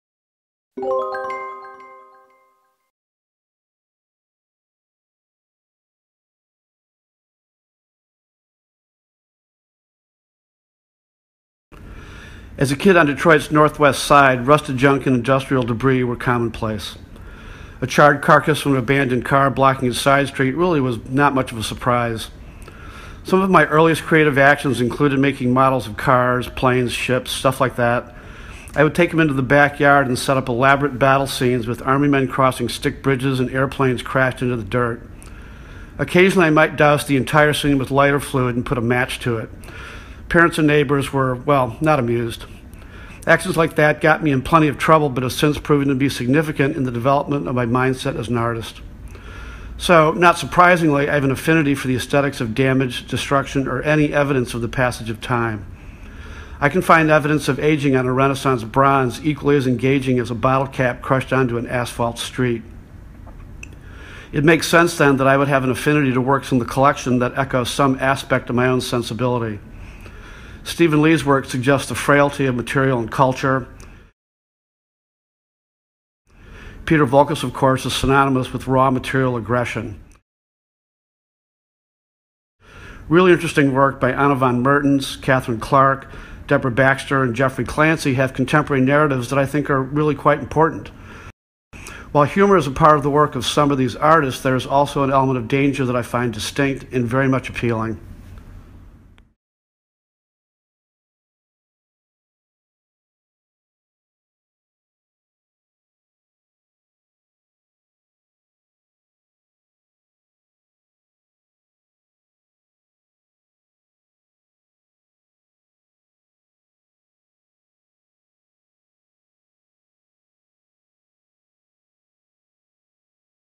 Steven Montgomery on Steven Young Lee—Connections: Renwick Gallery
Detroit-native Steven Montgomery discusses the influence of damage, destruction, and the passage of time in his own works, and finds similar narratives in the works of Steven Young Lee and Peter Voulkos, and other artists in the collection.